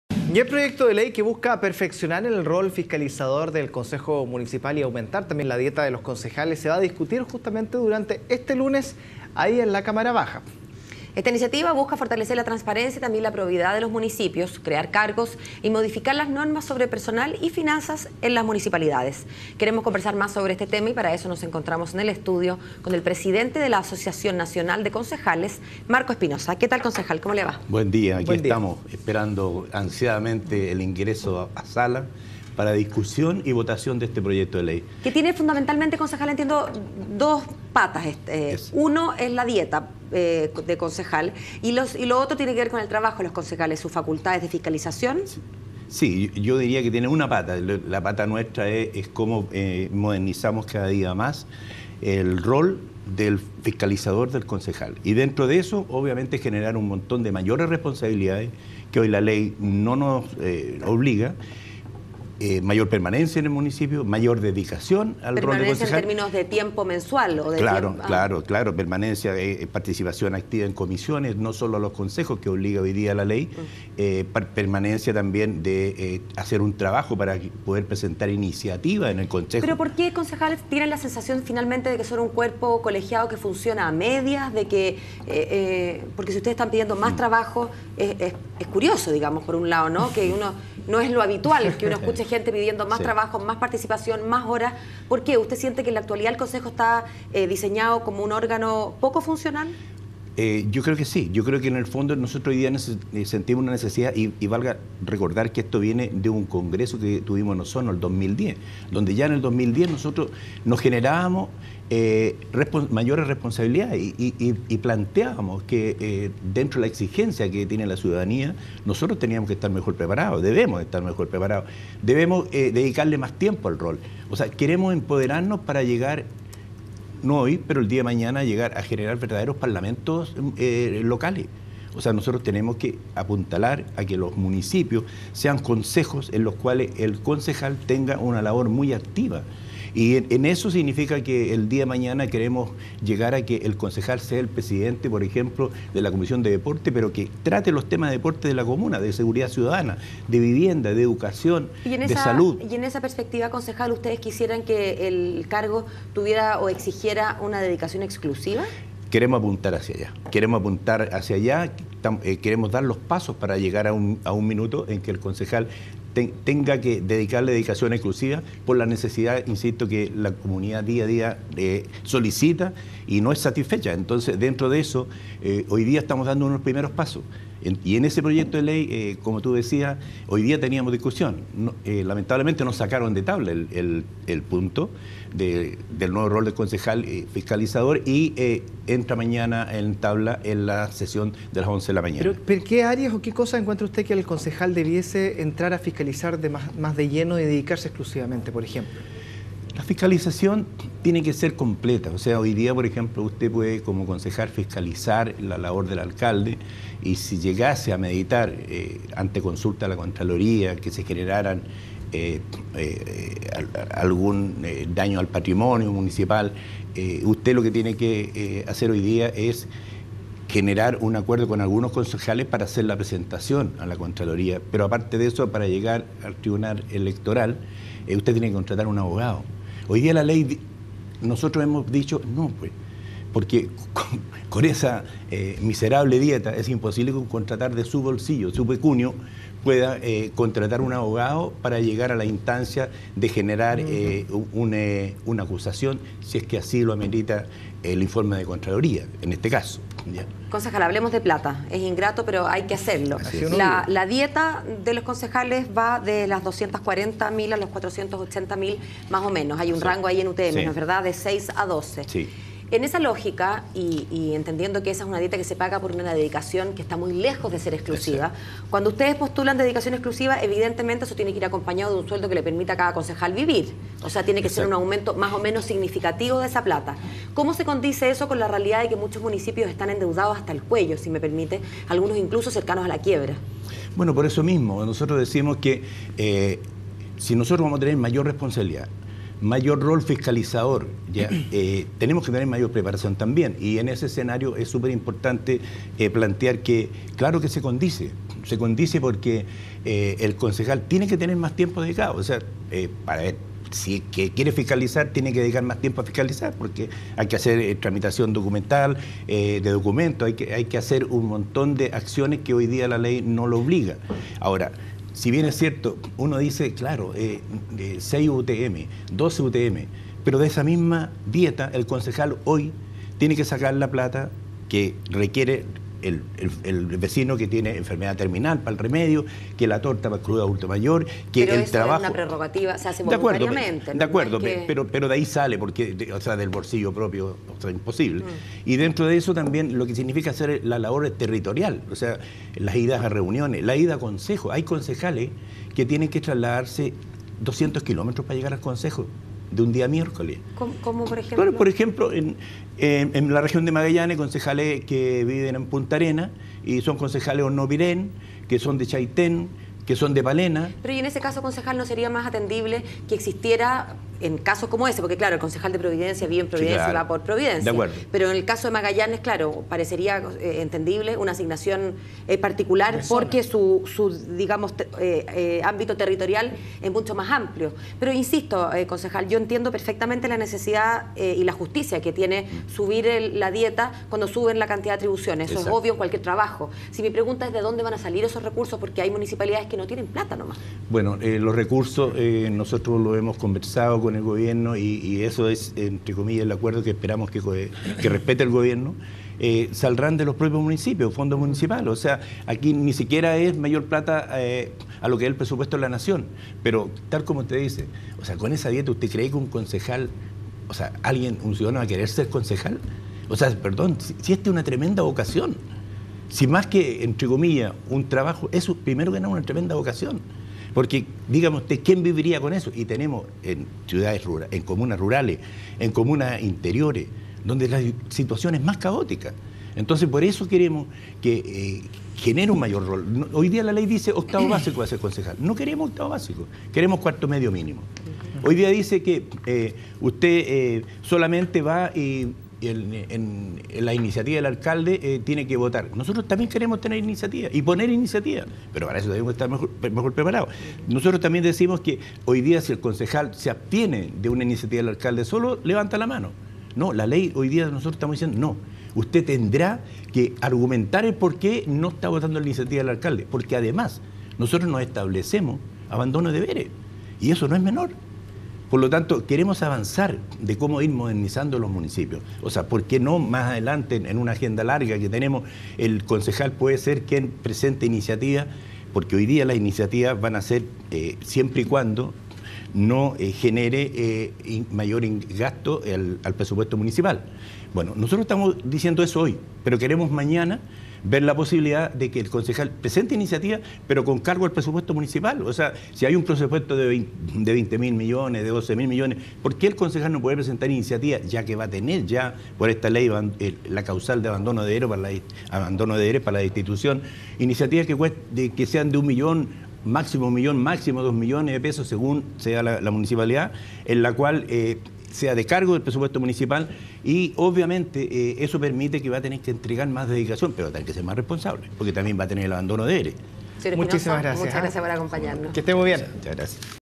Concejales piden un rol más fiscalizador y mejorar su dieta
Este lunes se discutirá el proyecto en la Cámara de Diputados.